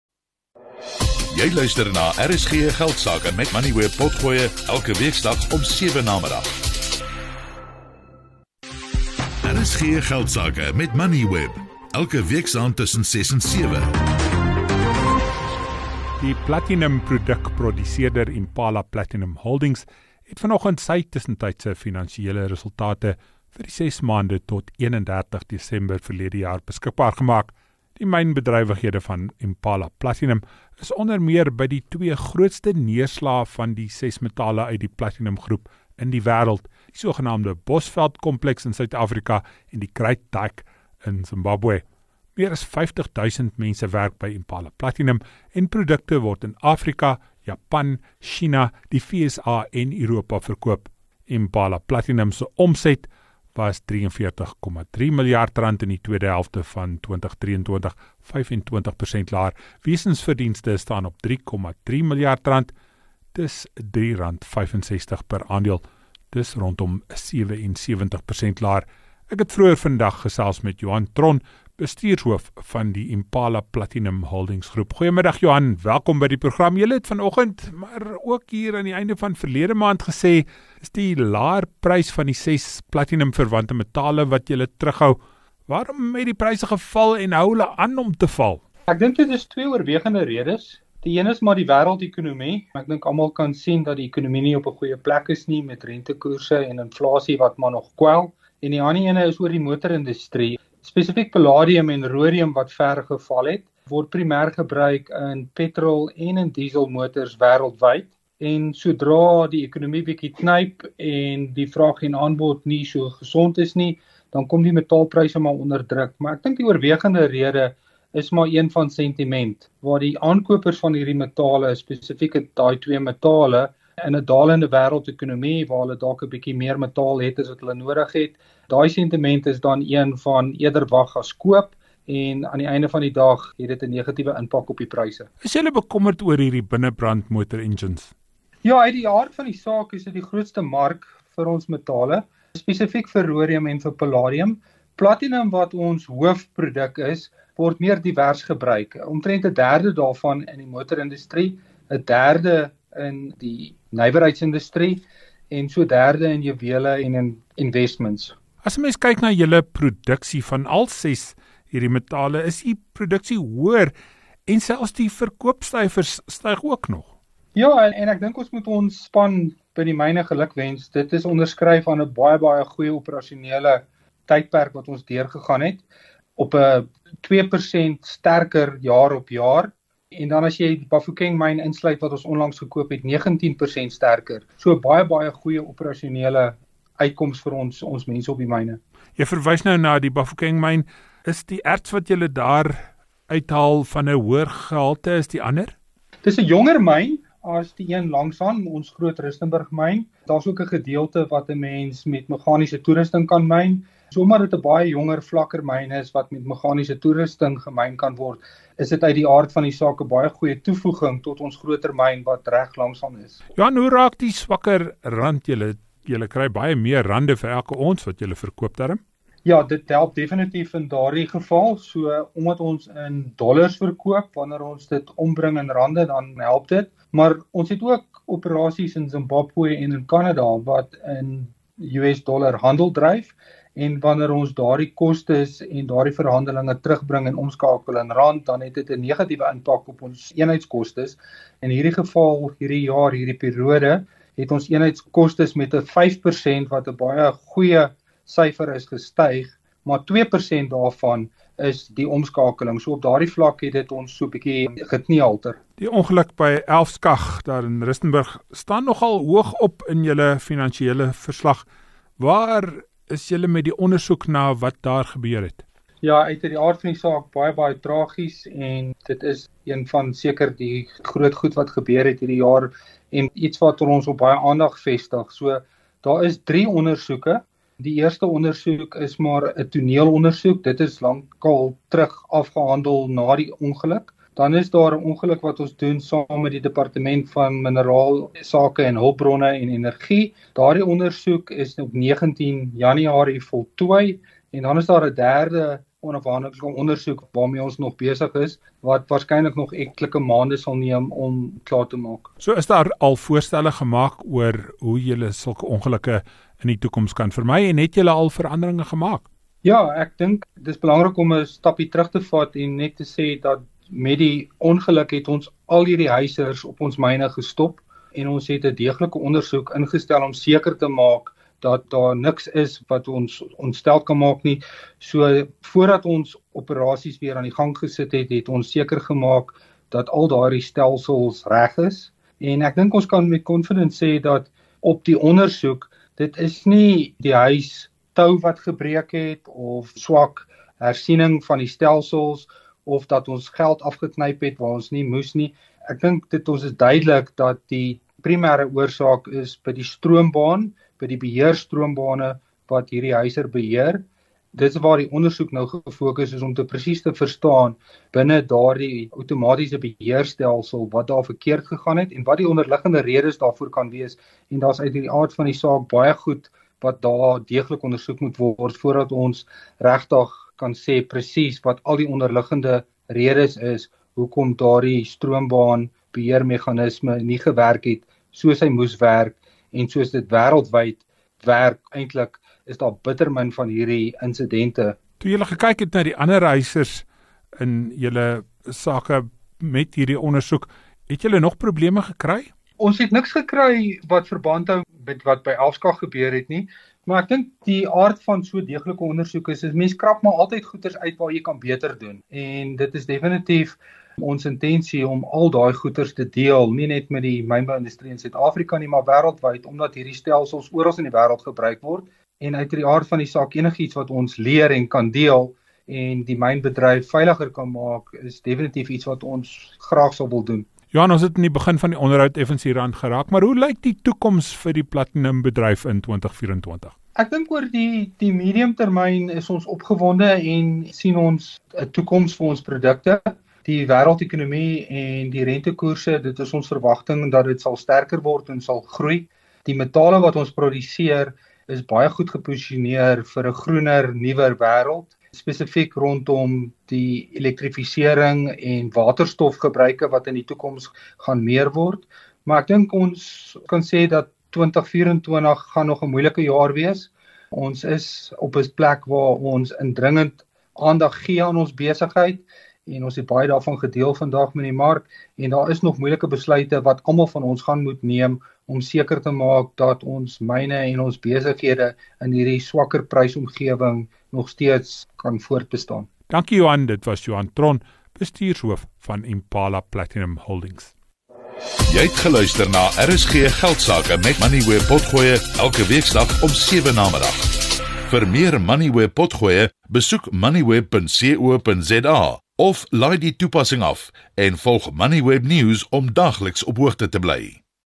Die swak platinumprys kou aan Impala Platinum se winste
Johan Theron, bestuurshoof van die Platinum Holdings Groep gesels oor hulle finansiële resultate. Volg RSG Geldsake op Twitter